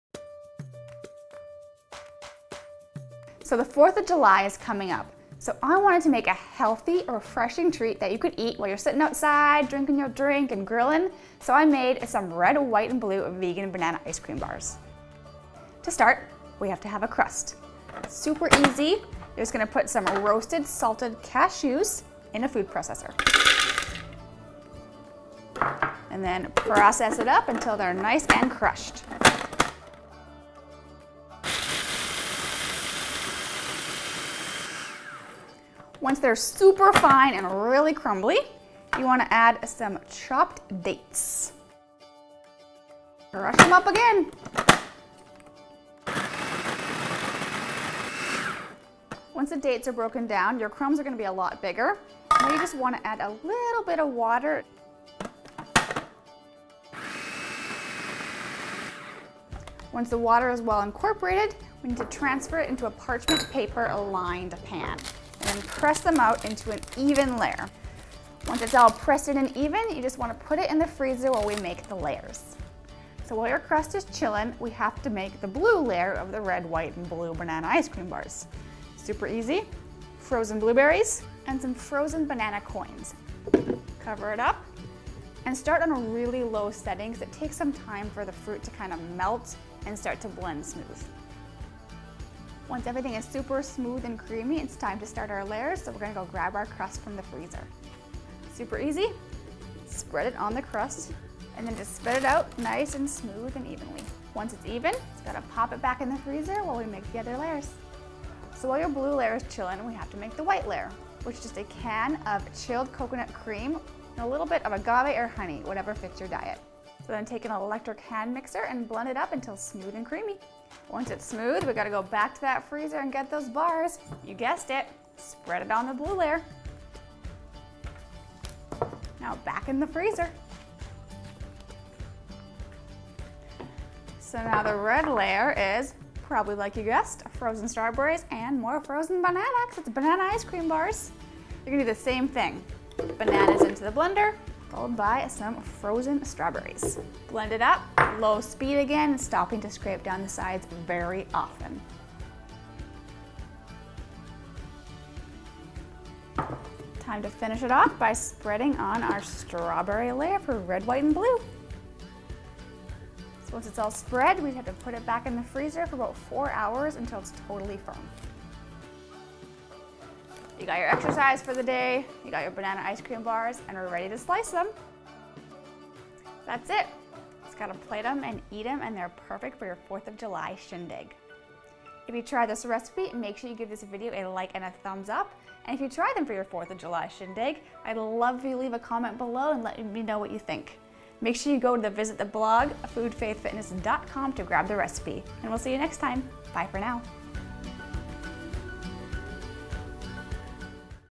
Red, White and Blue Banana Ice Cream Bars
How to make red, white and blue banana ice cream bars that are SO creamy! Gluten free, dairy free and so delicious!   Instagram Facebook

RECIPE:
INGREDIENTS:
For the crust:
2/3 Cup Roasted salted cashews* (93g)
1/2 Cup Dates, roughly chopped and loosely packed (75g )
1 tsp Water
For the bars:
2 Large Bananas, cut into coins and frozen overnight**
1 Cup Frozen Blueberries
1 Can Coconut cream (about 1 1/4 cups)
2 Tbsp Honey (or agave for vegan)
1 1/2 Cups Halved frozen strawberries, lightly heaping

DIRECTIONS:
Line an 8x8 inch pan with parchment paper, leaving enough to hang over the sides to use as a handle.

In a large food processor, process the cashews until broken down. Add in the dates and process until a crumbly dough forms. Then, with the food processor running, add in the water and process until well combined. Your crust should be sticky.

Press the crust evenly into the bottom of the prepared pan, making sure to pack it tight. Place into the freezer.

Place one of the banana coins, along with the blueberries, into a high-powered blender*** (I use a Blendtec) and blend on the chop setting until crushed. Then, go back and forth between the "chop" and "mix" setting, scraping the sides as necessary until well mixed and creamy. You will need to stop your blender A LOT, and very frequently, to scrape the sides in order to get everything mixed.

Spread the blueberry mixture over the prepared crust and place it back into the freezer to harden.

While the blueberry layer chills, scrape the coconut cream into a large bowl (don't add the coconut water!) along with the agave. Use an electric hand mixer to blend until light and fluffy, and no lumps remain.

Spread the coconut cream over the blueberry layer and let chill until firm to the touch, about 15-20 minutes.

Once the coconut layer is hard, add the remaining banana and strawberries to the blender and blend just like the blueberry layer. Again, you'll need to stop your blender A LOT to scrape down the sides to get it all mixed.

Spread the strawberry layer evenly over the coconut layer. Cover and refrigerate for at least 4 hours up to overnight.

When ready to slice, let the bars sit on the counter for 5-10 minutes to lightly soften. Then slice and DEVOUR.

NOTES:
*I buy mine pre-roasted. If yours aren't, put them in a 350 degree oven for 10-12 minutes.

**Because you use the bananas separately, I like to freeze the coins in separate bags, so I know exactly how much is one banana!

***I high-powered food processor works too. Just let the fruit sit our for 10-15 mins or so, to soften it

I think the bars are best served if they've sat out a few minutes, to let the coconut cream soften a little bit.